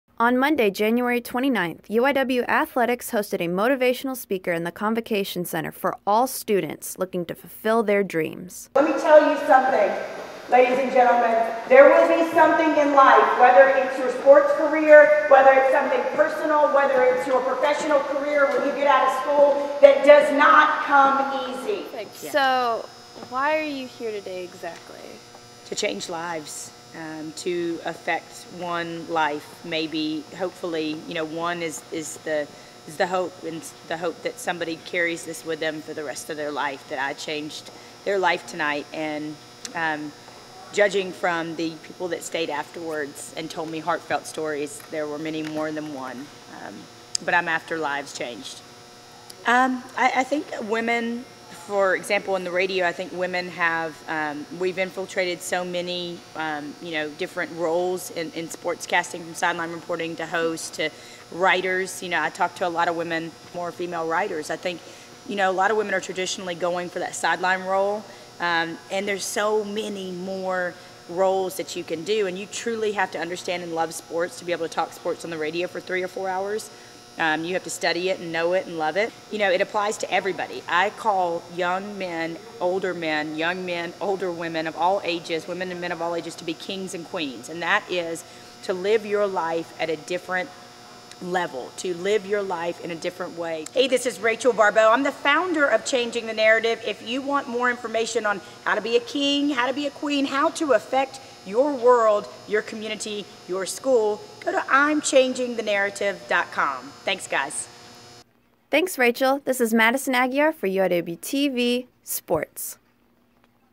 Student Athletes Welcome Rachel Baribeau
UIW invited special guest, Rachel Baribeau, to talk to student athletes about the hardships of life and how to overcome them. Want to learn how to be a King or Queen? Follow her on twitter @RachelBaribeau and check out her